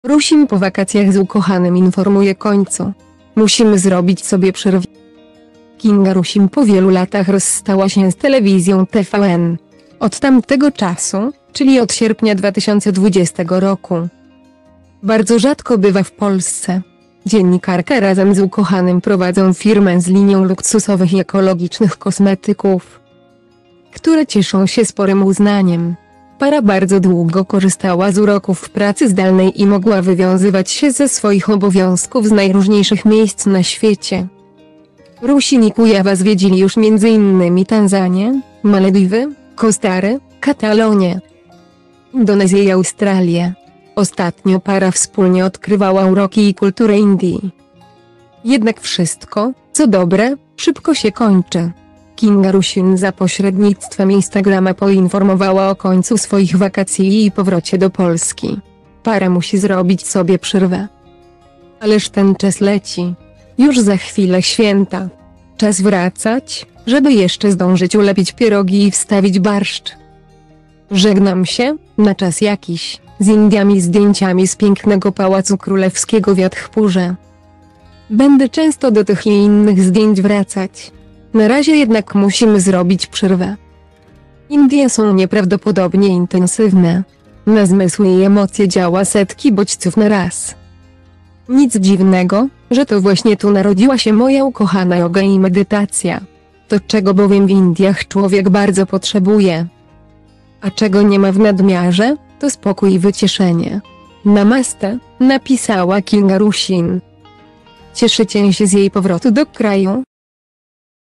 Rusin po wakacjach z ukochanym informuje końcu! "Musimy zrobić sobie PRZERWĘ"!
Kinga Rusin po wielu latach rozstała się z telewizją TVN. Od tamtego czasu, czyli od sierpnia 2020 roku,bardzo rzadko bywa w Polsce. Dziennikarka razem z ukochanym prowadzą firmę z linią luksusowych i ekologicznych kosmetyków,które cieszą się sporym uznaniem. Para bardzo długo korzystała z uroków pracy zdalnej i mogła wywiązywać się ze swoich obowiązków z najróżniejszych miejsc na świecie.